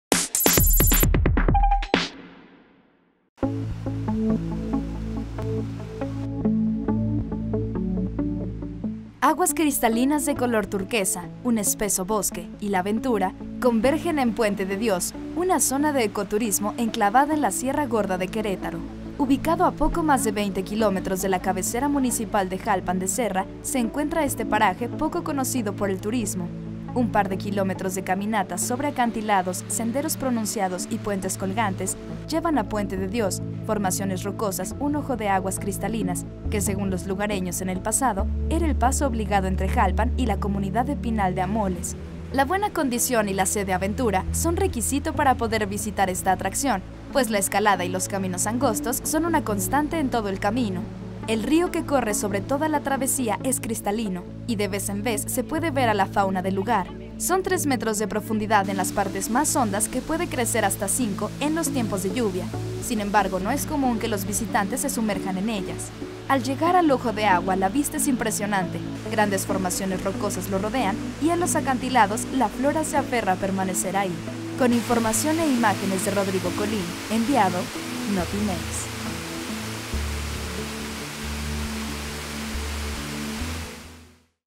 Puente de Dios, ecoturismo en Sierra Gorda
Formaciones rocosas y acantilados dan vida a Puente de Dios, un punto de ecoturismo en Jalpan de Serra